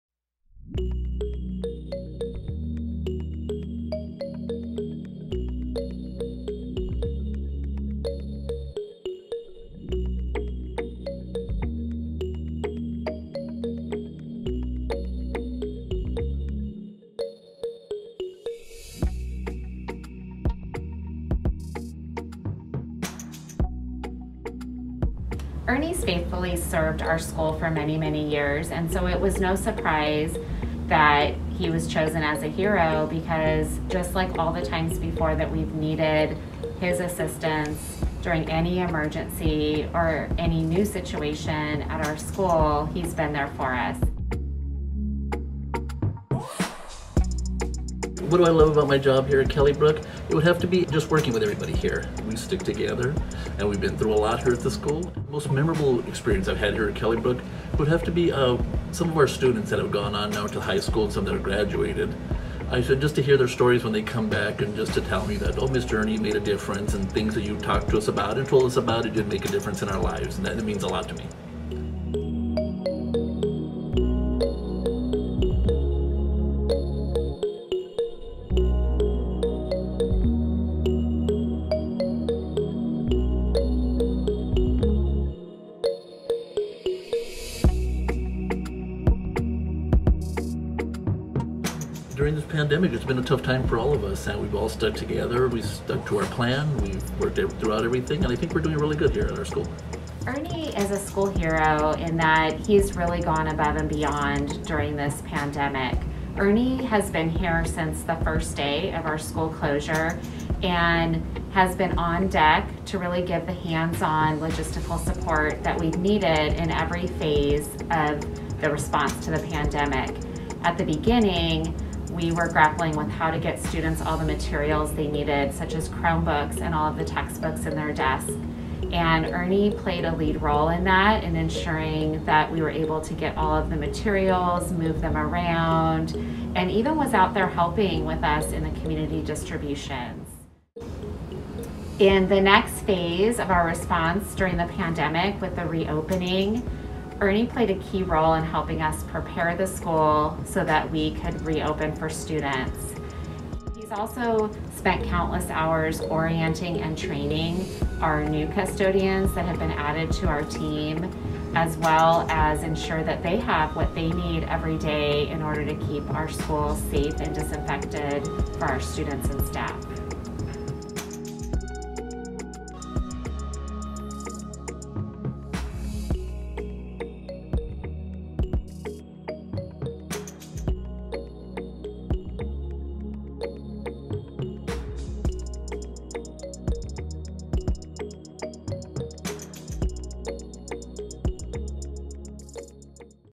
School Hero | Custodian Ernie Perez
Newport-Mesa Unified School District (NMUSD) is grateful to all of our employees who have worked tirelessly throughout the pandemic to support a safe and productive learning environment.

This short video highlights one of our MANY amazing employees that are positively impacting the lives of students, families, and colleagues every day.

Our first School Hero is Killybrooke Elementary Custodian, Ernie Perez. One of our many dedicated custodians who keep our schools and District facilities clean and safe!